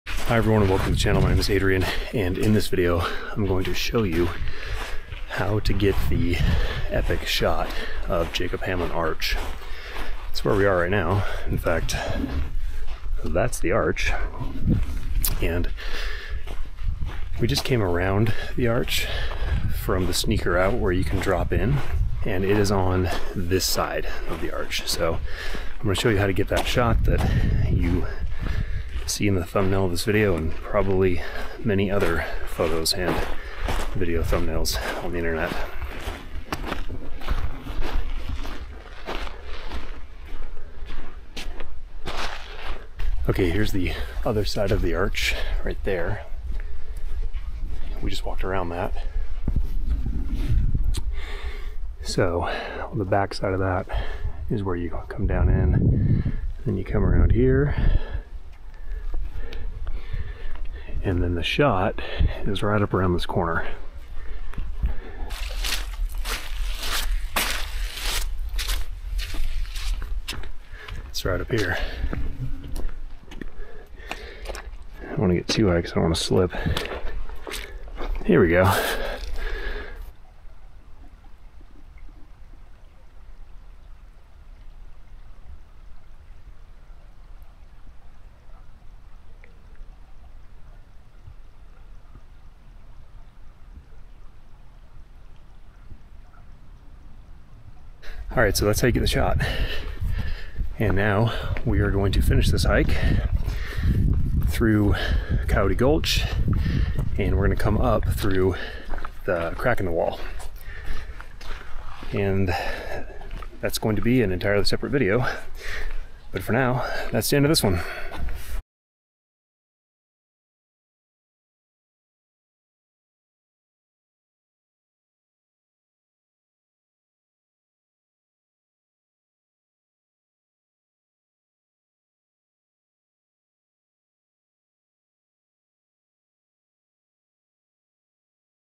How to get the epic shot of Jacob Hamblin Arch in Coyote Gulch
In this video, I show you how to get the epic shot of Jacob Hamblin Arch in Coyote Gulch.

USEFUL COORDINATE
--- 37.39251, -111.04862 Jacob Hamblin trailhead.
--- 37.39251, -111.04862 Approximate rappel point.
--- 37.41900, -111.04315 Jacob Hamblin Arch.